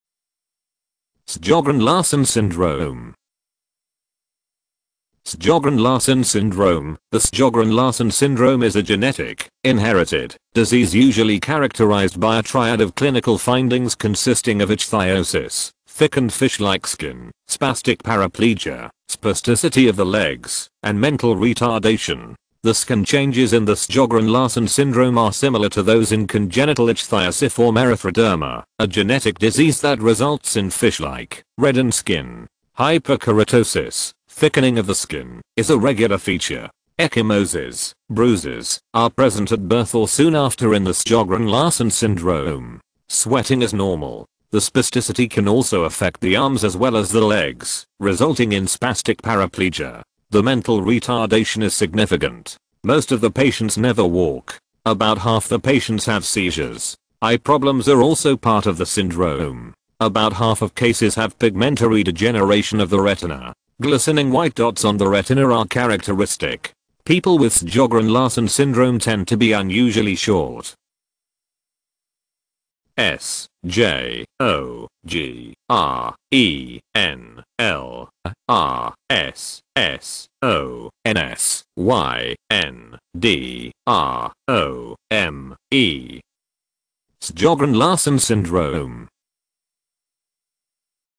Sjogren-Larsson syndrome - Medical Meaning and Pronunciation
Title: Sjogren-Larsson syndrome

Sjogren-Larsson syndrome: The Sjogren-Larsson syndrome is a genetic (inherited) disease usually characterized by a triad of clinical findings consisting of ichthyosis (thickened fish-like skin), spastic paraplegia (spasticity of the legs) and mental retardation. The skin changes in the Sjogren-Larsson syndrome are similar to those in congenital ichthyosiform erythroderma, a genetic disease that results in fish-like, reddened skin. Hyperkeratosis (thickening of the skin) is a regular feature. Ecchymoses (bruises) are present at birth or soon after in the Sjogren-Larsson syndrome. Sweating is normal. The spasticity can also affect the arms as well as the legs, resulting in spastic paraplegia. The mental retardation is significant. Most of the patients never walk. About half the patients have seizures. Eye problems are also part of the syndrome. About half of cases have pigmentary degeneration of the retina. Glistening white dots on the retina are characteristic. People with Sjogren-Larsson syndrome tend to be unusually short.